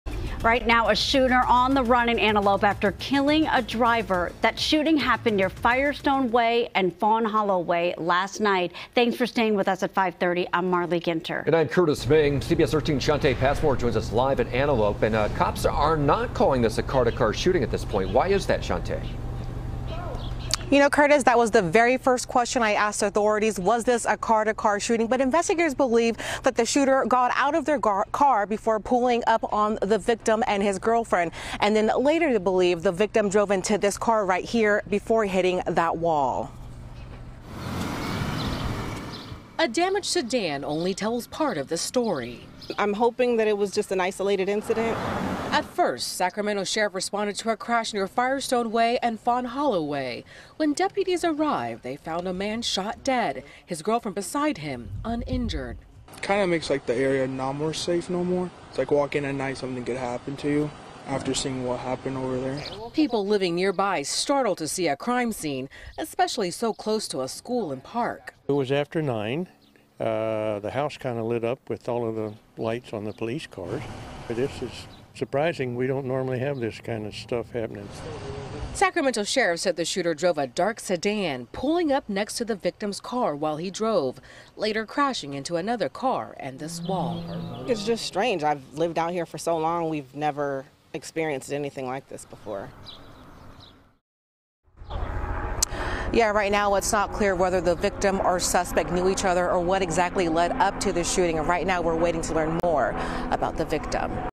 Antelope homicide raises concerns while shooter on the run
A gunman is on the run after shooting and killing a driver Monday night.